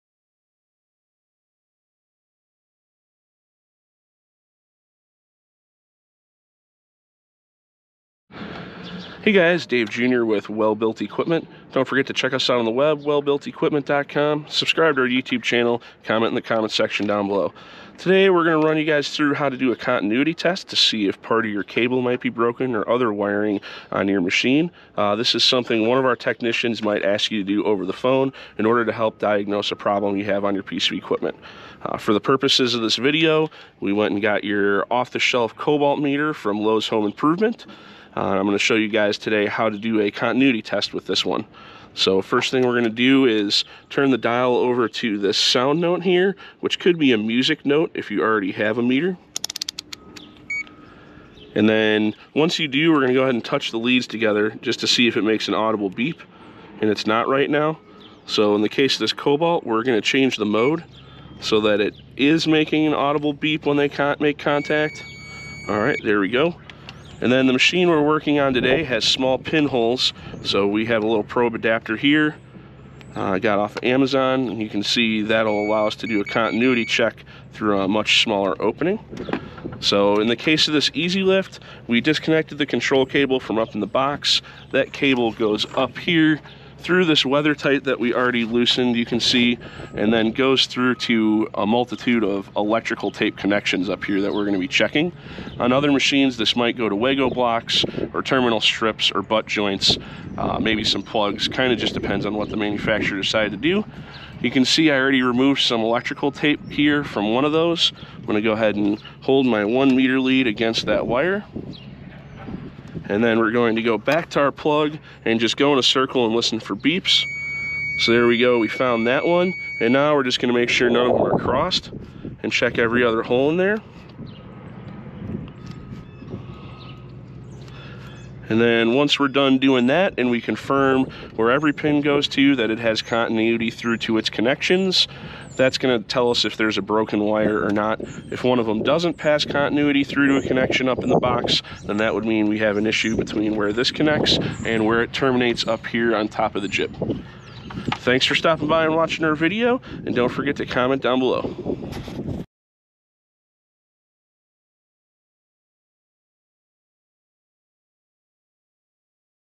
Multimeter Basics EP1 - Continuity Testing for Damaged Wiring & Cables on Equipment
Our first episode in a series of how to videos for basic diagnosing of issues with your machine features Dave Jr going through a test for continuity with a multi meter. This is a procedure that one of our technicians may have you perform on your machine while performing a remote repair.